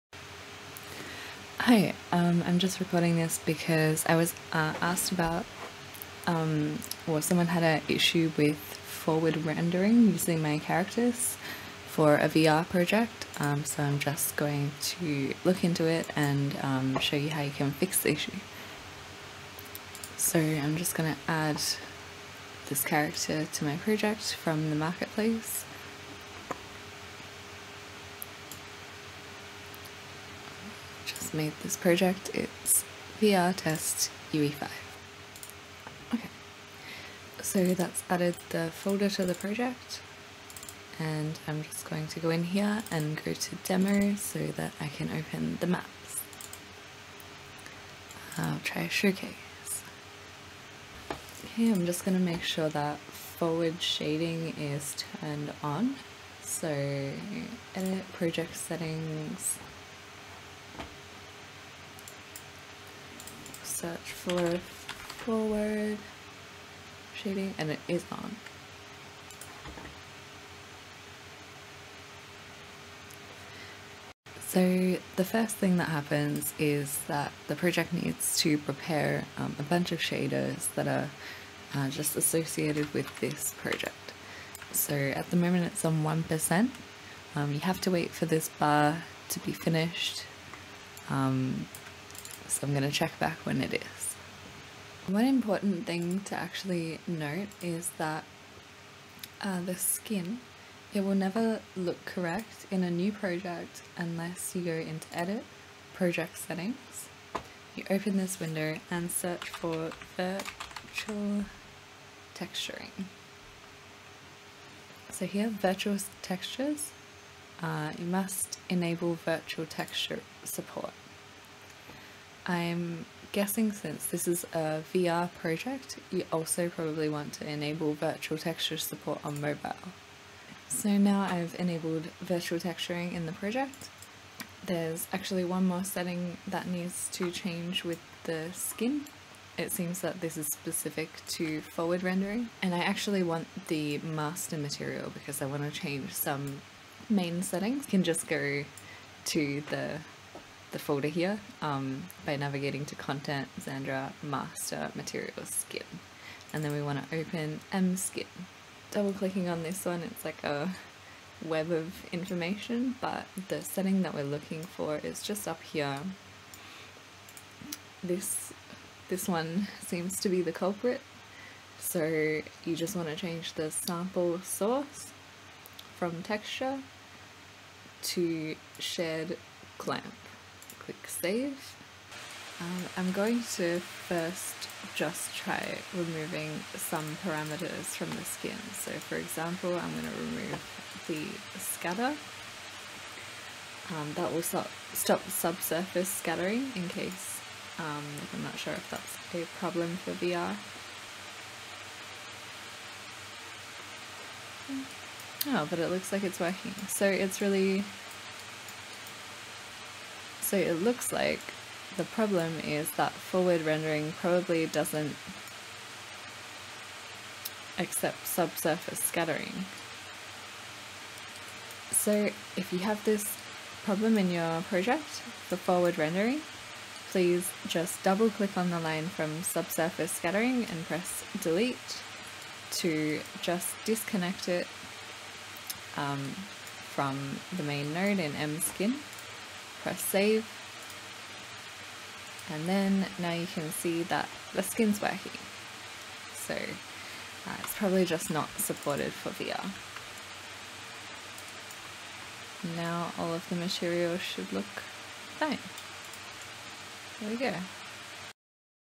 XANDRA Custom Characters for Unreal VR (Forward Rendering)
This video shows the required steps to use my characters with Forward Rendering in Unreal Engine!